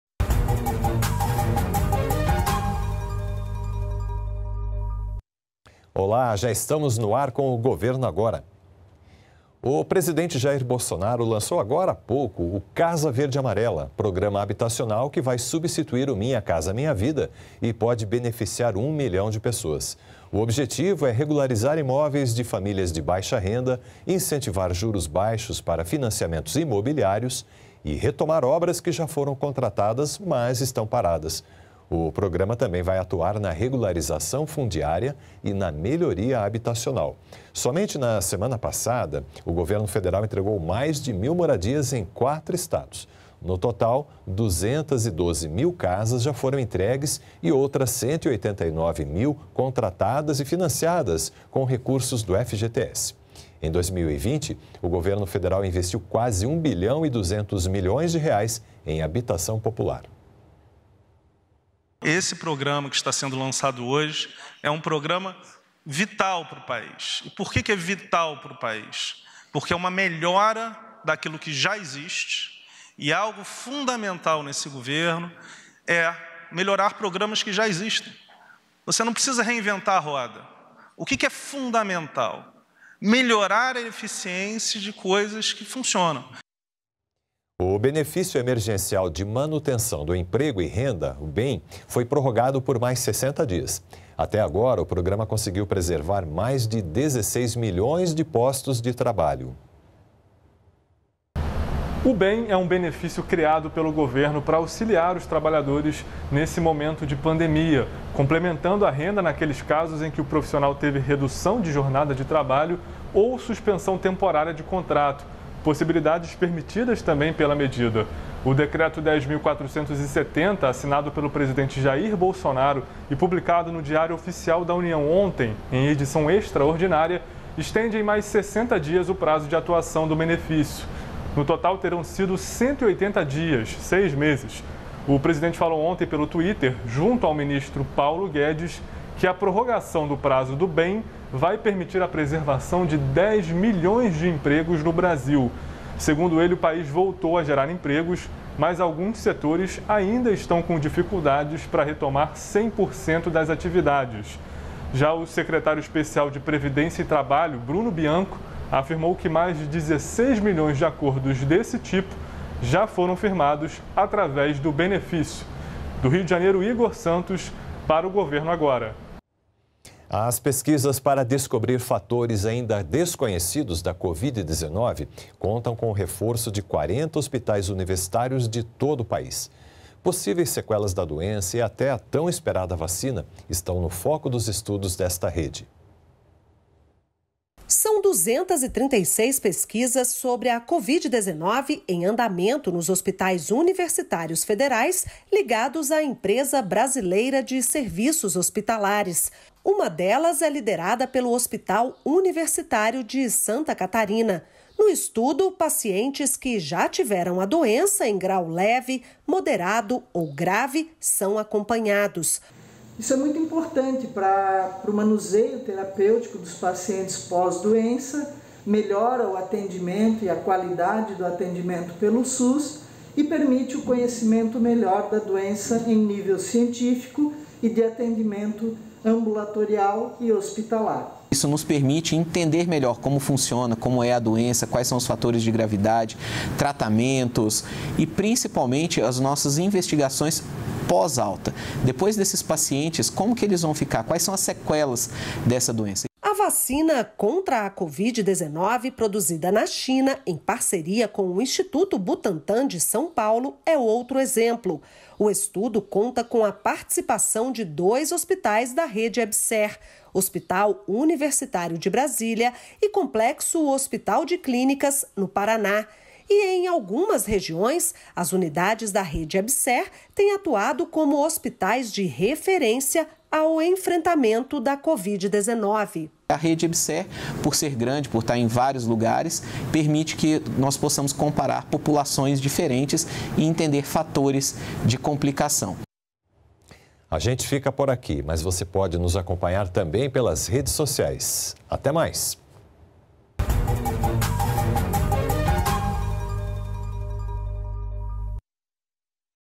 Programa Casa Verde e Amarela pode beneficiar mais de 1 milhão de pessoas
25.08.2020 - Foi lançado nesta terça-feira (25) o programa habitacional Casa Verde e Amarela, que pode beneficiar 1 milhão de pessoas. Objetivo é levar moradias para famílias de baixa renda. Benefício Emergencial de Manutenção do Emprego e Renda é prorrogado por mais 60 dias. Pesquisas sobre Covid-19 reúnem 40 hospitais universitários de todo o país.